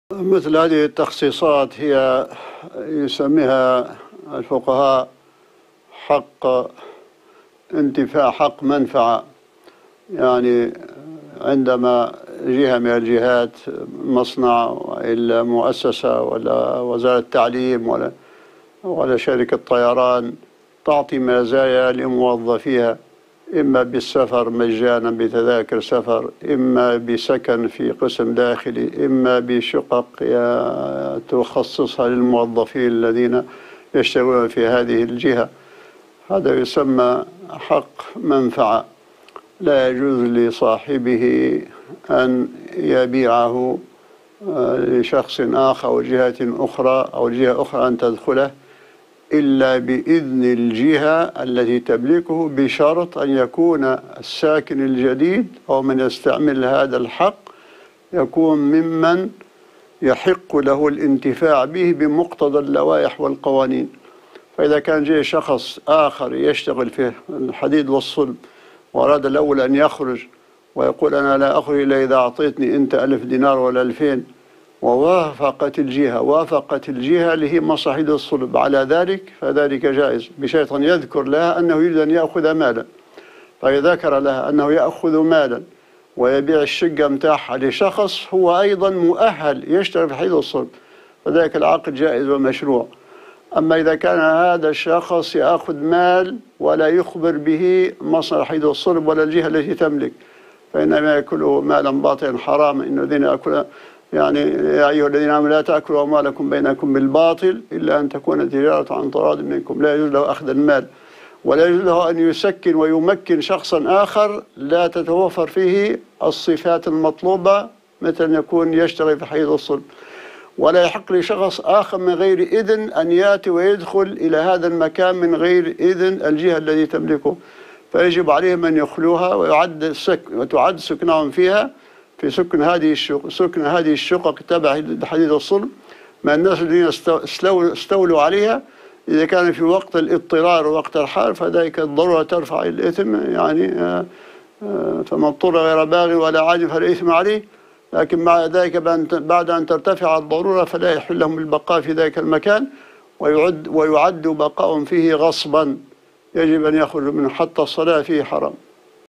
الشيخ د.الصادق الغرياني | حكم التصرف في مساكن خاصة بالشركات واستعمالها بلا إذن
الشيخ د.الصادق الغرياني | حكم التصرف في مساكن خاصة بالشركات واستعمالها بلا إذن | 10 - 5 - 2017

ــــــــــــــــــــــــــــــــــــــــــــــــــــــــــــــــــــــــــ
ــــــــــــــــــــــــــــــــــــــــــــــــــــــــــــــــــــــــــ
يمكنكم تحميل تطبيقنا TANASUH TV على الهواتف الذكية: